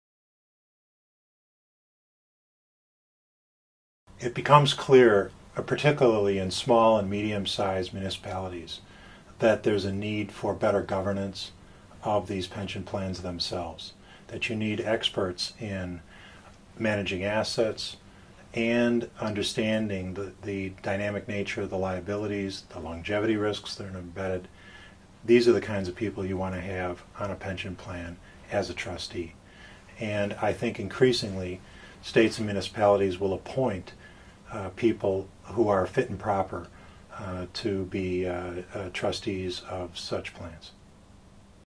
U.S. public pension crisis - governance
John Fitzpatrick, Secretary General of The Geneva Association, on the issue of governance of U.S. public pension plans in tackling the funding crisis.

While there have been several municipal bankruptcies over time, on 3 December 2013, Detroit became the largest city in the U.S. to be legally eligible for Chapter 9 bankruptcy. Its ballooning deficits and large pension shortfall are characteristic of municipal bankruptcy cases. Across the country, states have posted pension funding shortfalls of over a trillion dollars. A report published by The Geneva Association, "The Public Pensions Crisis in the U.S." proposes that non-payment of annual required contributions and an unbalanced structure to pension planning are among the most significant challenges to overcome.